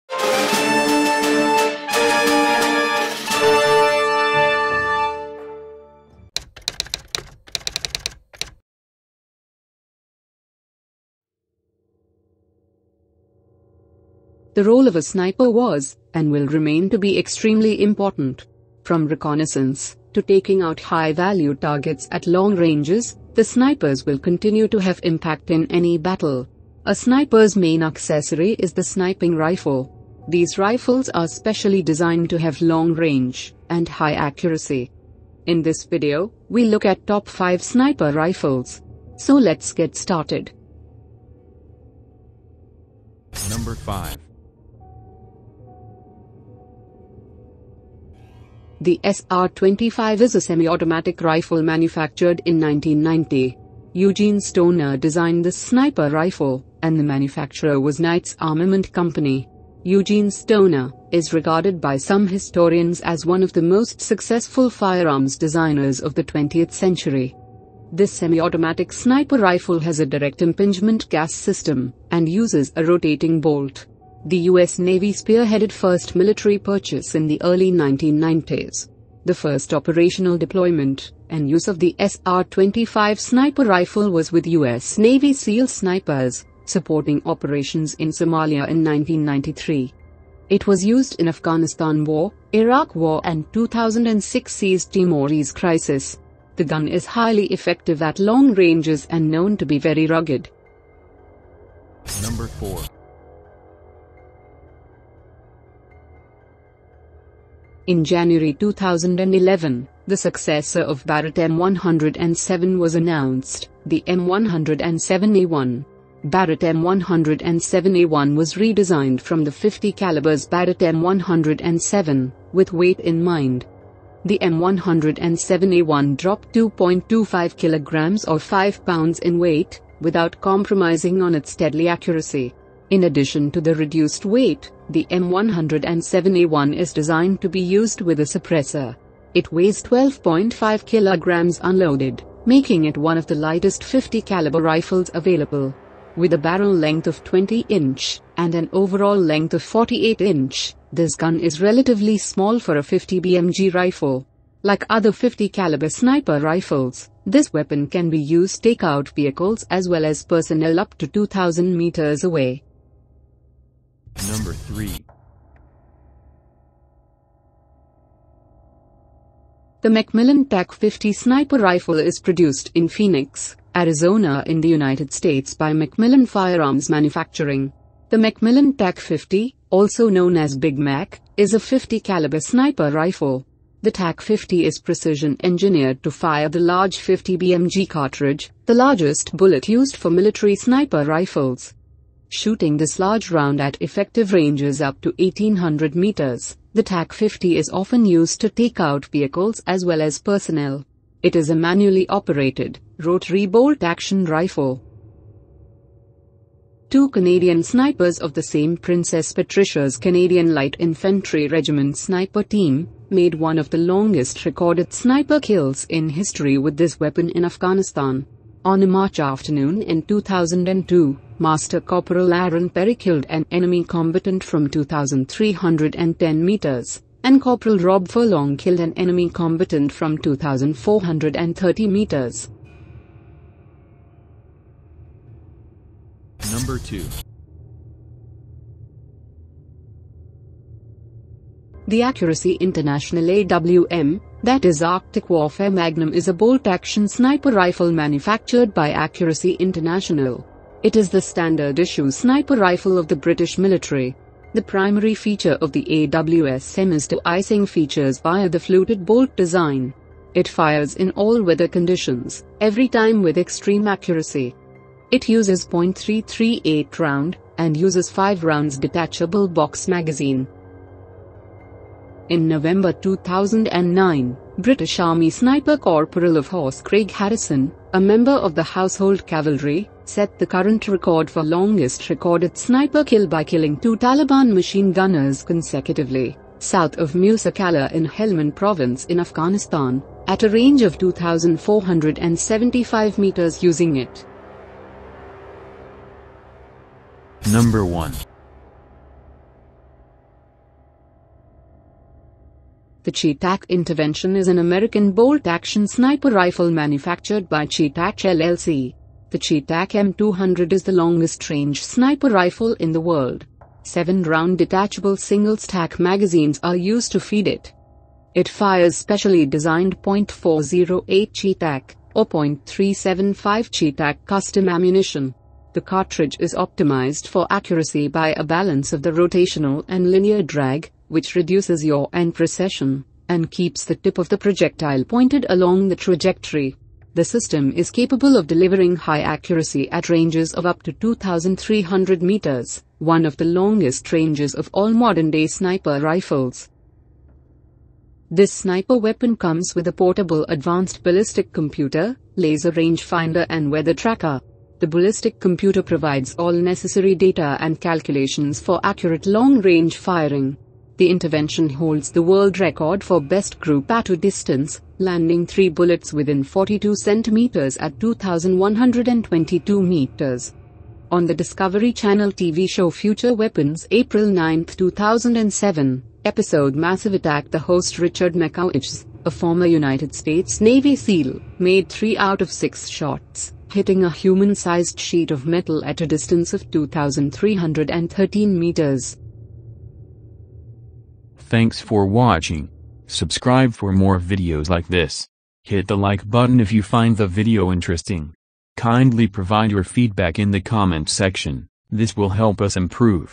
TOP 5 SNIPER RIFLES OF THE WORLD
The role of a sniper was, and will remain to be extremely important. From reconnaissance, to taking out high value targets at long ranges, the snipers will continue to have impact in any battle.
A snipers main accessory is the sniping rifle. These rifles are specially designed to have long range, and high accuracy.  In this video, we look at top 5 sniper rifles. So lets get started.

SR-25:

The SR-25 is a semi-automatic rifle manufactured in 1990. Eugene Stoner designed this sniper rifle, and the manufacturer was Knight’s Armament Company.

Eugene Stoner, is regarded by some historians as one of the most successful firearms designers of the 20th century.

This semi-automatic sniper rifle has a direct impingement gas system, and uses a rotating bolt.

The U.S. Navy spearheaded first military purchase in the early 1990s. The first operational deployment, and use of the SR-25 sniper rifle was with U.S Navy SEAL snipers, supporting operations in Somalia in 1993.

It was used in Afghanistan War, Iraq War and 2006 East Timorese Crisis.  The gun is highly effective at long ranges and known to be very rugged.

BARRETT M107A1

In January 2011, the successor of Barrett M107 was announced, the M107A1.

Barrett M107A1 was redesigned from the 50 calibers Barrett M107, with weight in mind.

The M107A1 dropped 2.25 kg or 5lbs in weight, without compromising on its deadly accuracy. In addition to the reduced weight, the M107A1 is designed to be used with a suppressor.

It weighs 12.5 kg (27.4 lbs) unloaded, making it one of the lightest 50 caliber rifles available. With a barrel length of 20 inch, and an overall length of 48 inch, this gun is relatively small for a 50 BMG rifle.

Like other 50 caliber sniper rifles, this weapon can be used take out vehicles as well as personnel up to 2,000 meters away.

MCMILLAN TAC 50

The McMillan Tac-50 sniper rifle is produced in Phoenix, Arizona in the United States by McMillan Firearms Manufacturing.

The Mcmillan TAC 50, also known as “Big Mac”, is a 50-caliber sniper rifle.
The TAC 50 is precision engineered to fire the large 50 BMG cartridge, the largest bullet used for military sniper rifles. Shooting this large round at effective ranges up to 1800 meters, the TAC 50 is often used to take out vehicles as well as personnel.
It is a manually operated, rotary bolt-action rifle.

2 Canadian snipers of the same Princess Patricia's Canadian Light Infantry (PPCLI) regiment sniper team, made one of the longest recorded sniper kills in history with this weapon in Afghanistan.
On a March afternoon in 2002, Master Corporal Arron Perry killed an enemy combatant from 2310 meters, and Corporal Rob Furlong killed an enemy combatant from 2430 meters.

Accuracy International AWM

The Accuracy International AWM, that is Arctic Warfare Magnum is a bolt-action sniper rifle manufactured by Accuracy International.
It is the standard issue sniper rifle of the British Military.
The primary feature of the AWSM is de-icing features via the fluted bolt design. It fires in all weather conditions, every time with extreme accuracy. It uses .338 round, and uses 5 rounds detachable box magazine.

In November 2009, British Army sniper Corporal of Horse (CoH) Craig Harrison, a member of the Household Cavalry, set the current record for longest recorded sniper kill by killing two Taliban machine gunners consecutively, south of Musa Qala in Helmand Province in Afghanistan, at a range of 2475 m (2,707 yd) using it.

CHEYTAC M200 INTERVENTION

The CheyTac Intervention is an American bolt-action sniper rifle manufactured by CheyTac LLC.  The CheyTac M200 is the longest-range sniper rifle in the world.
7 round detachable single-stack magazines are used to feed it. It fires specially designed  .408 Chey Tac , or .375 Chey Tac custom ammunition.

The cartridge is optimized for accuracy by a balance of the rotational and linear drag, which reduces yaw and precession, and keeps the tip of the projectile pointed along the trajectory.
The system is capable of delivering high accuracy at ranges of up to 2300 m, one of the longest ranges of all modern-day sniper rifles. This sniper weapon comes with a portable advanced ballistic computer, laser rangefinder and weather tracker. The ballistic computer provides all necessary data and calculations for accurate long range firing.

The Intervention holds the world record for best group at a distance, landing 3 bullets within 42 cm at 2122 m.

On the Discovery Channel TV show Future Weapons April 9, 2007, episode "Massive Attack" the host Richard Machowicz, a former United States Navy SEAL, made 3 out of 6 shots, hitting a human-sized sheet of metal at a distance of 2,313 m.